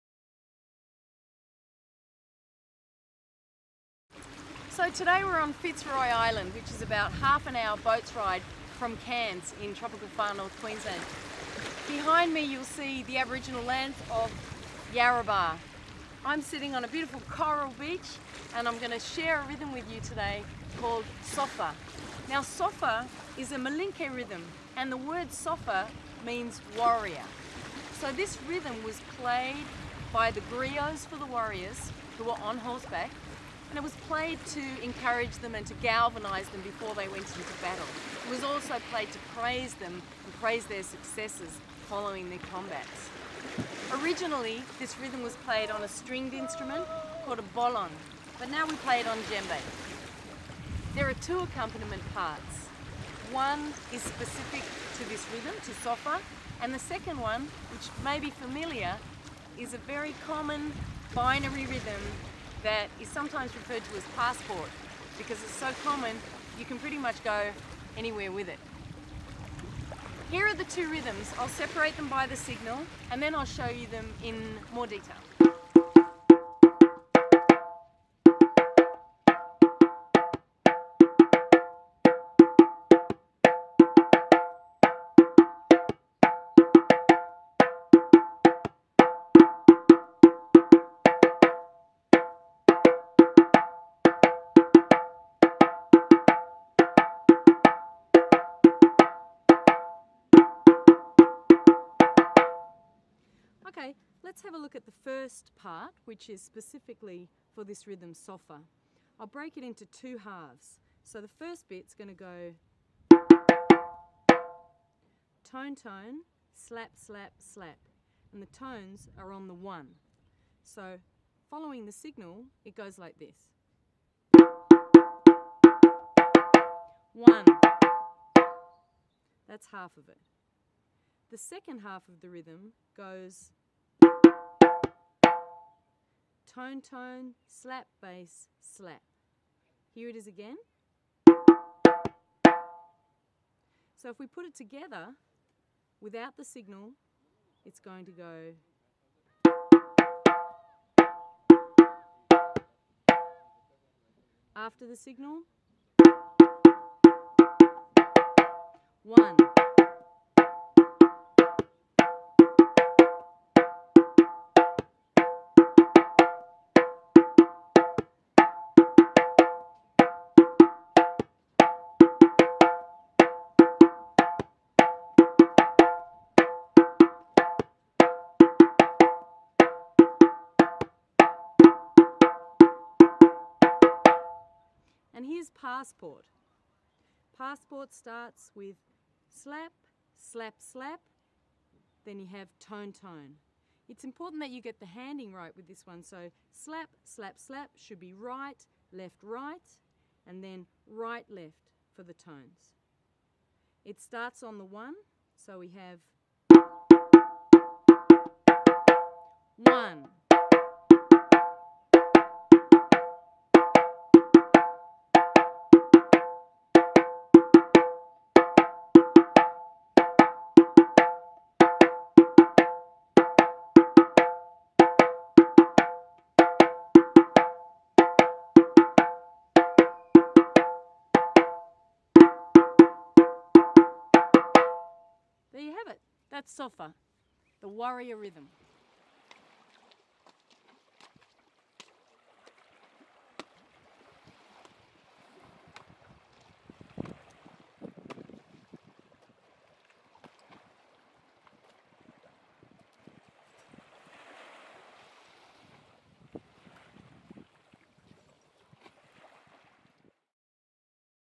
Drum Up Big - demonstration of Sofa djembe accompaniments
Tara Tucker demonstrates 2 djembe accompaniment parts for the Malinke rhythm, Sofa.  Filmed by Kim Arrow at Fitzroy Island, Queensland, Australia.